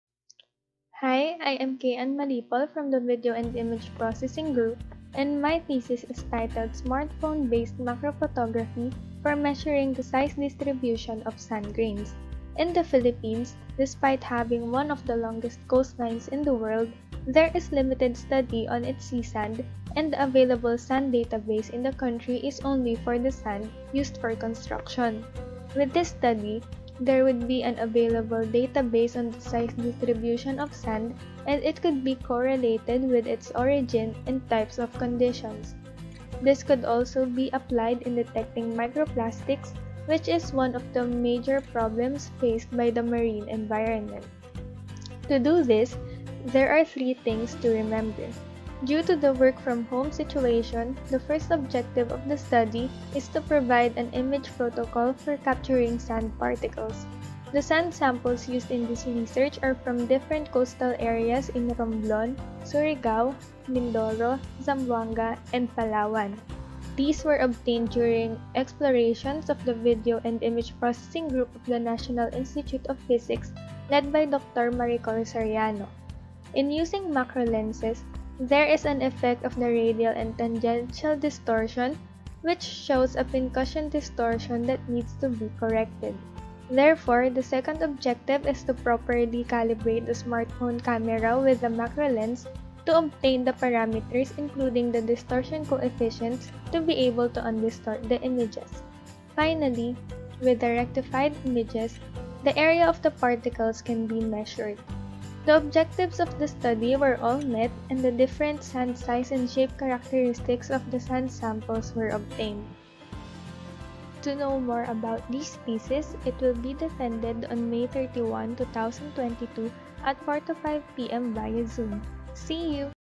Smartphone-based macro photography for measuring the size distribution of sand grains [CAV Malipol]
31 May 2022 (Tuesday)
4:00 - 5:00 PM

B.S. Applied Physics Thesis Defense
Chae Ann V. Malipol

Title: Smartphone-based macro photography for measuring the size distribution of sand grains

Adviser: Maribor N. Soriano, Ph.D.
Examiners: Jayson Cabanilla, Christian Ray Buco